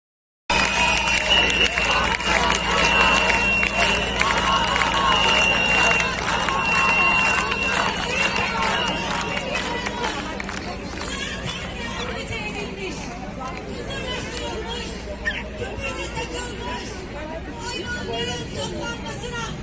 Hundreds of people gathered in Kadıköy for stray animals
Hundreds of people gathered in Kadıköy for stray animals: “Shelters are death camps”

In Istanbul Kadıköy, animal lovers protested against the confiscation of stray animals by municipalities.
Sokak hayvanları için yüzlerce kişi Kadıköy'de buluştu: “Barınaklar ölüm kamplarıdır”

İstanbul Kadıköy'de hayvanseverler, sokak hayvanlarının belediyeler tarafından toplatılmasını protesto etmek amacıyla eylem yaptı.